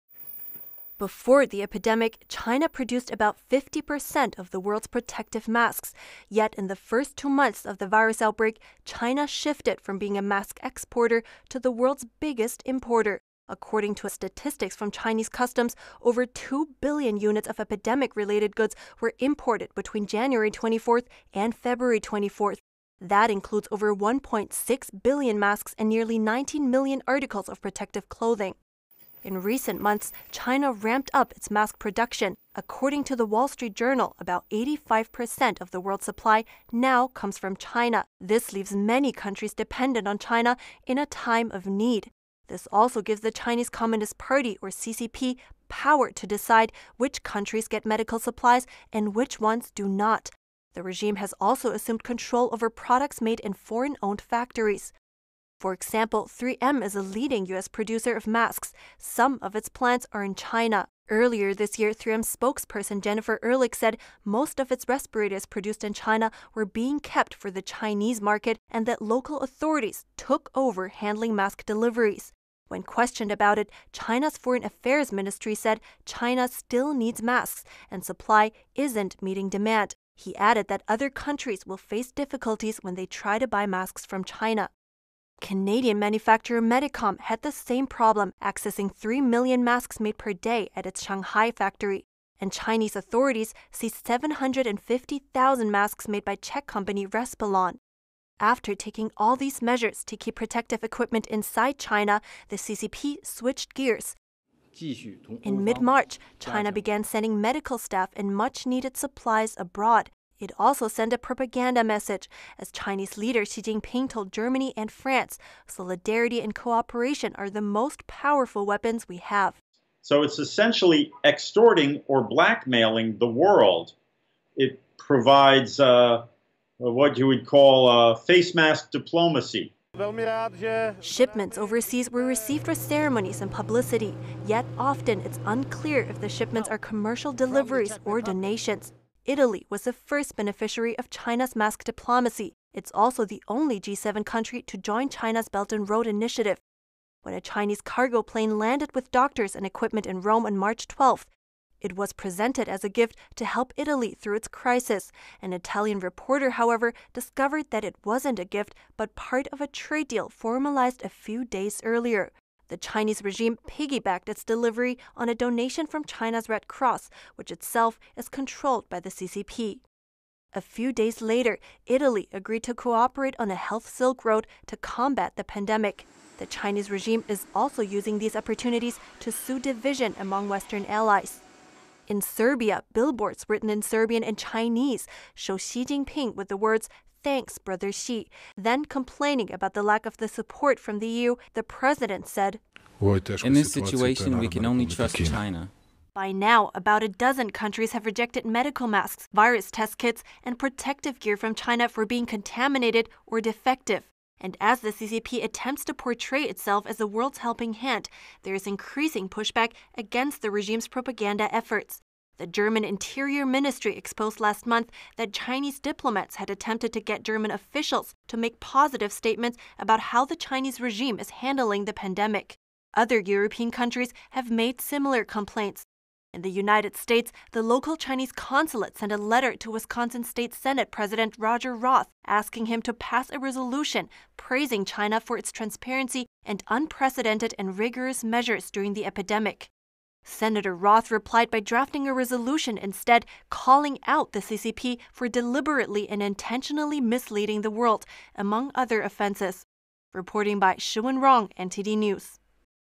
Chinese Regime’s Mask Diplomacy Strategy | NTD
Since the outbreak began, China has been hoarding medical supplies from around the world. Now that the CCPvirus has spread and countries need their own supplies.

Before the epidemic, China produced about 50% of the world's protective masks.